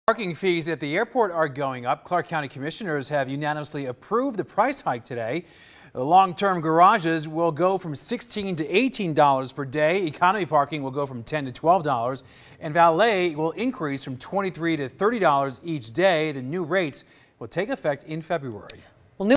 Airport parking price hike approved
Parking fees at McCarran Airport are going up. Clark County commissioners unanimously approved the price hike on Tuesday.